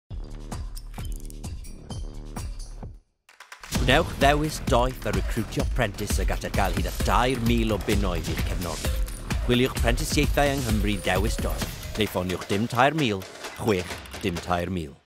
Gwnewch ddewis doeth a recriwtio prentis
Gwnewch ddewis doeth a recriwtio prentis a gallech gael hyd at dair mil o bunnoedd i’ch cefnogi.